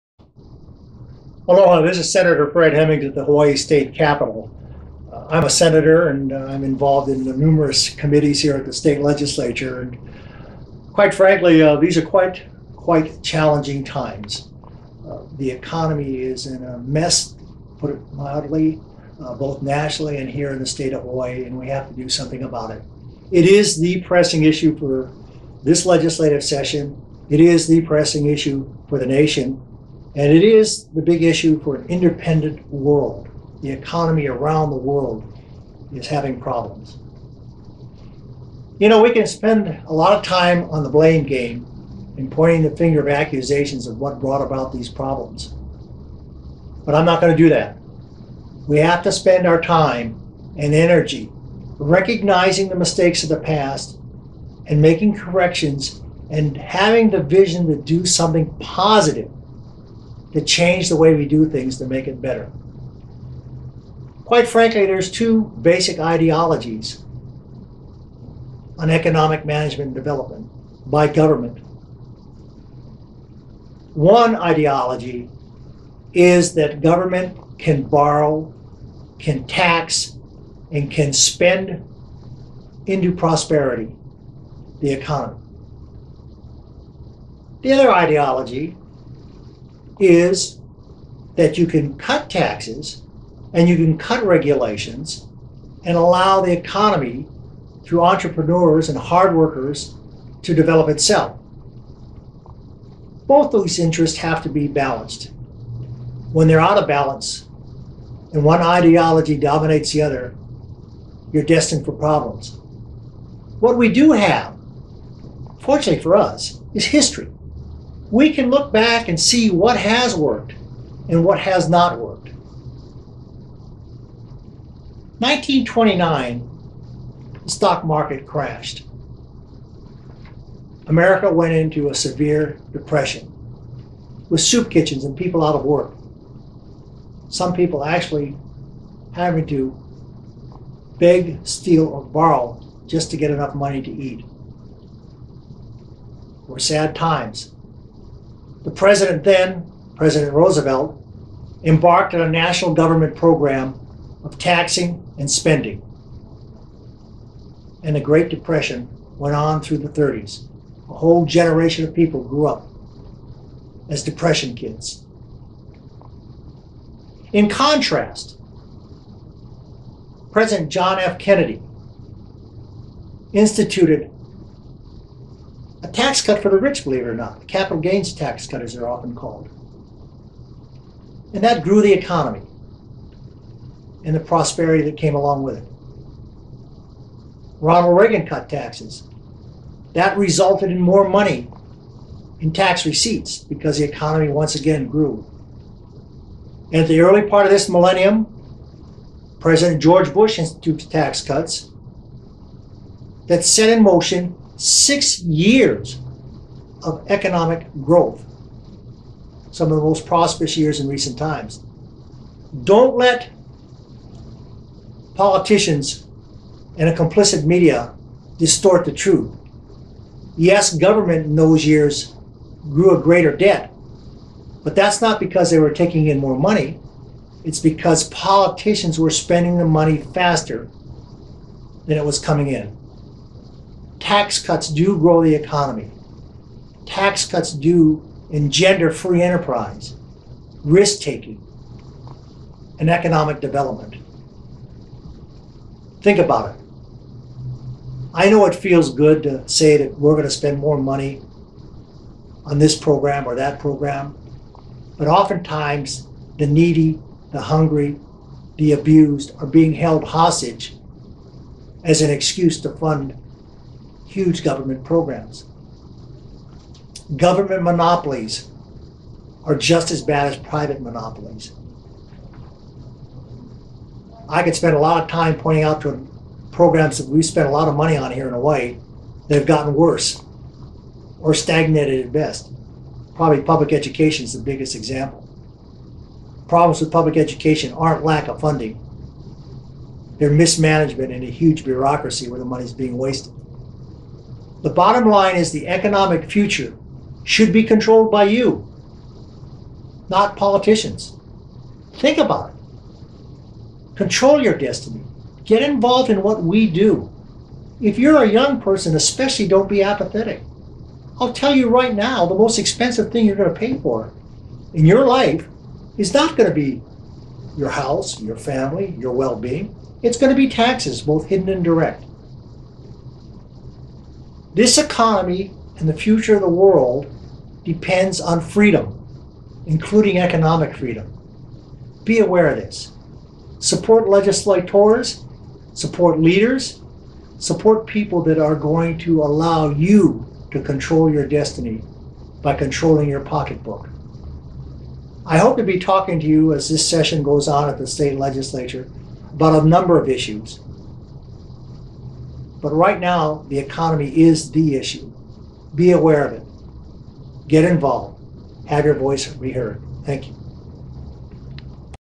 Senator Fred Hemmings-- February 9, 2009 Legislative Update
Senator Fred Hemmings offers his insight on these economically challenging times.